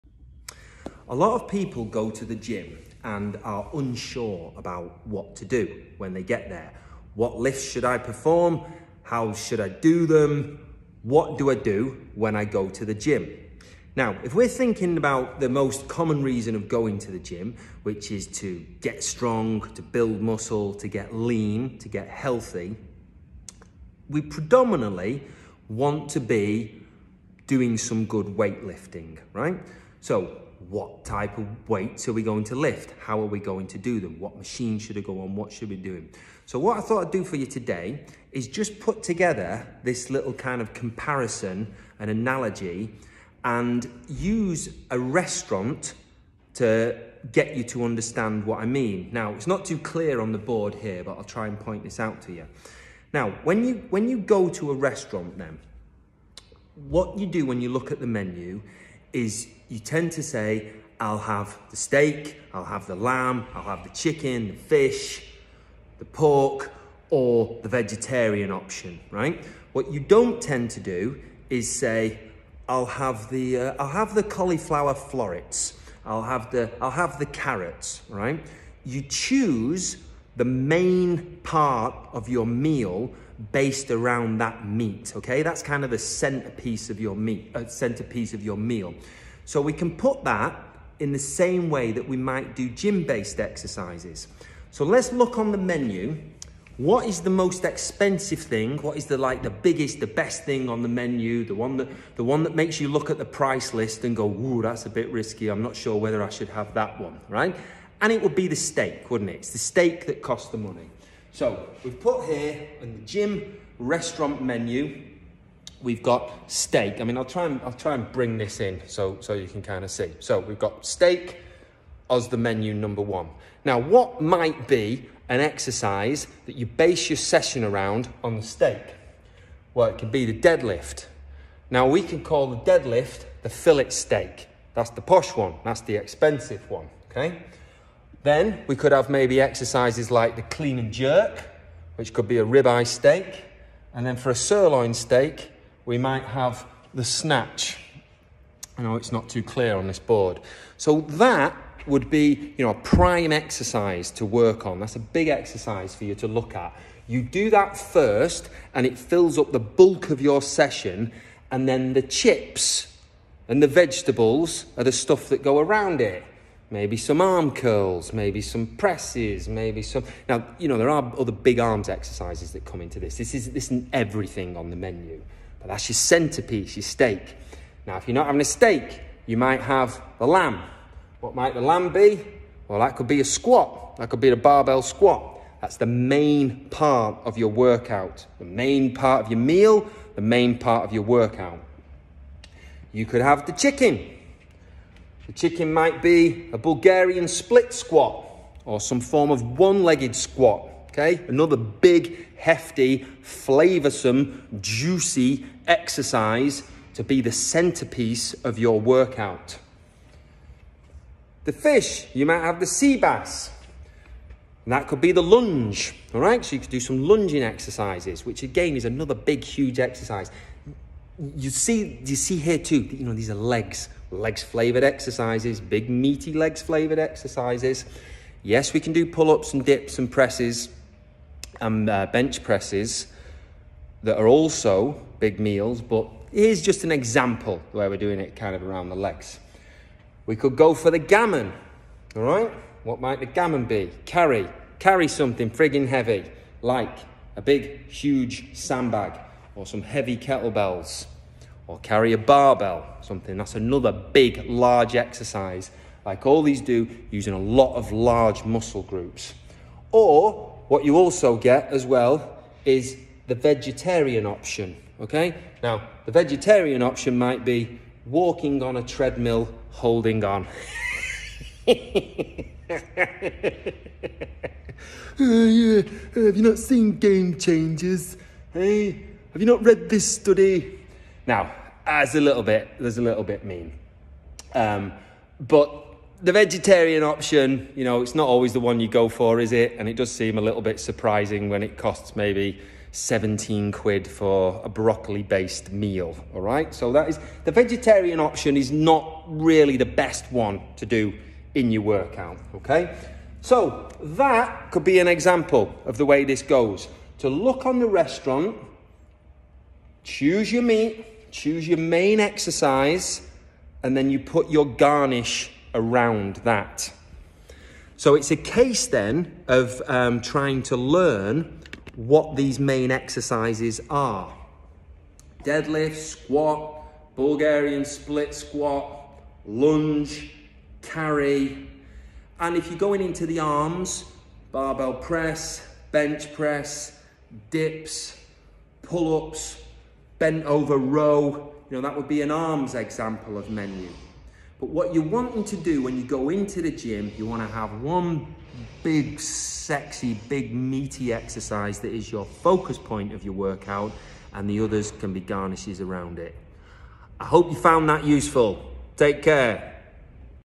Workouts and Restaurant Menus
Understanding a restaurant menu may help you to construct a workout in the gym.